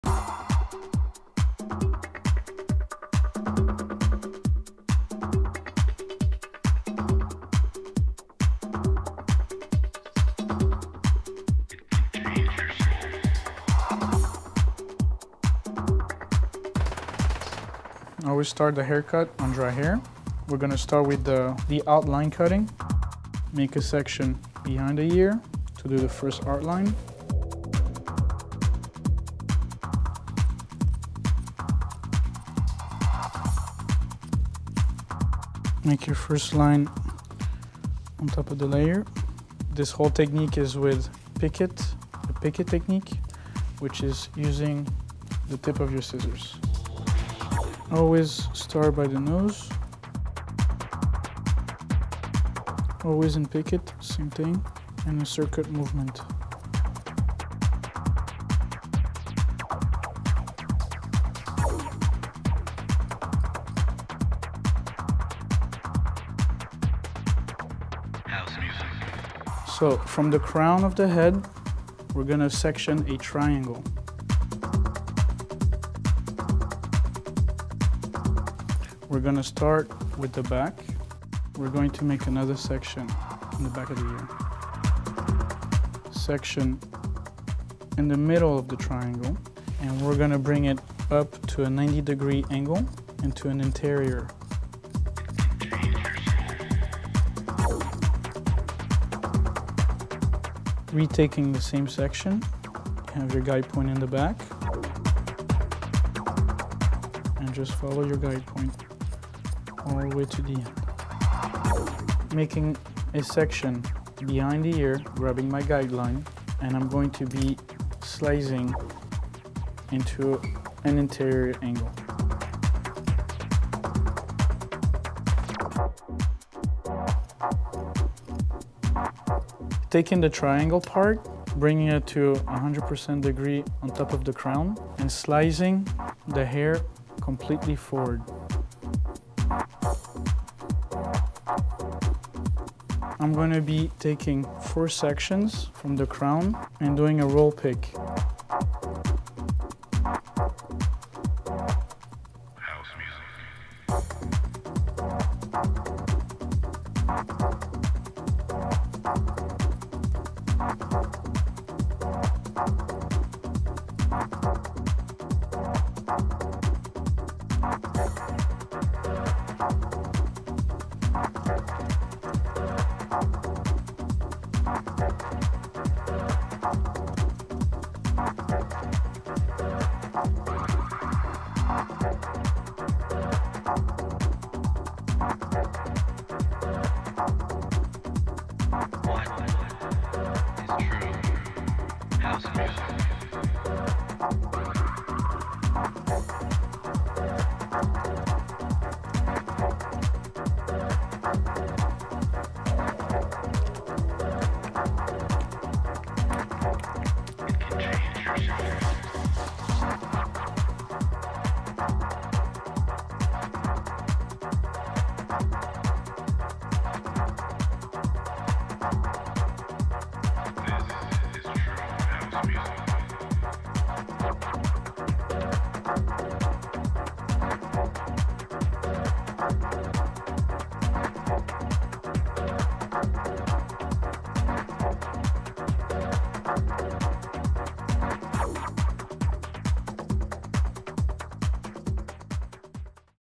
Miles of Hair Cutting - Hair Styling How To (Promotional)
Watch how to cut hair, learn how to cut bangs, learn how to cut short hair and short hair styles. GKhair Educator Fabrice Castellisi created the Miles of Hair Cut. Watch the exciting hair cutting video.